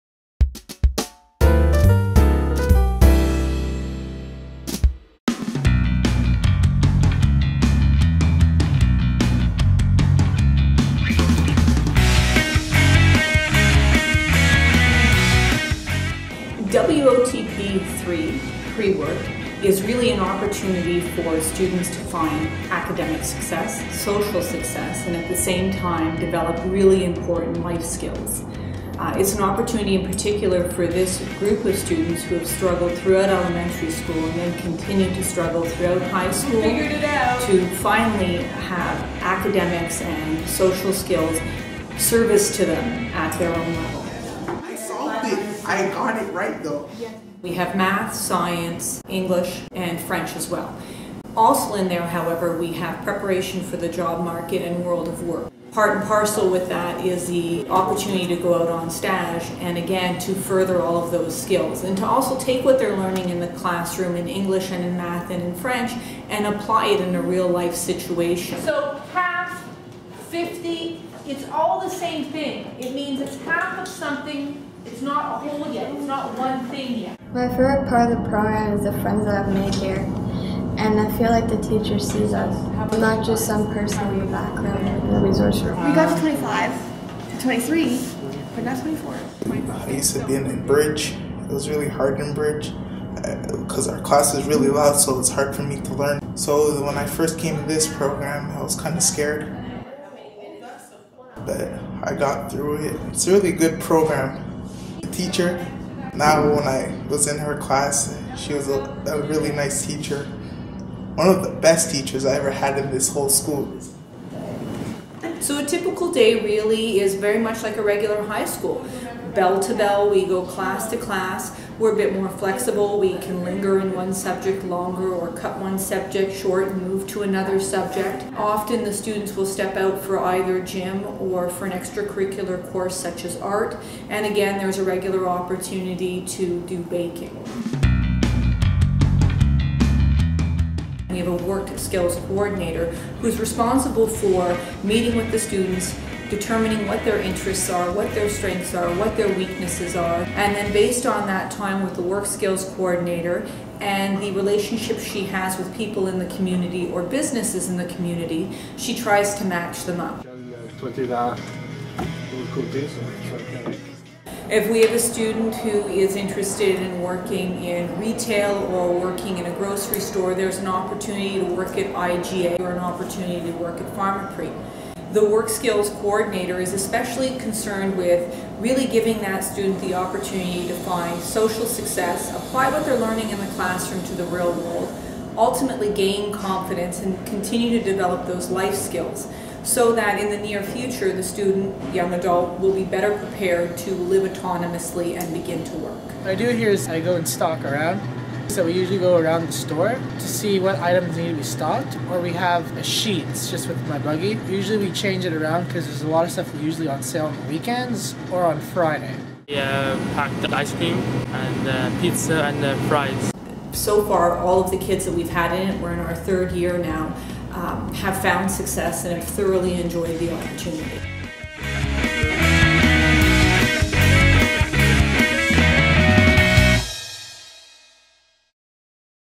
Work Oriented Training Path - Prework Training
A promotional video from Lester B. Pearson School Board.
Learn about the Work Oriented Training Program (WOTP).
The video promotes the "pre-work" program.

Directed and Edited by John Le Blanc - Educational Services, LBPSB
Filmed in cooperation with Pierrefonds Comprehensive High School and Student Services, LBPSB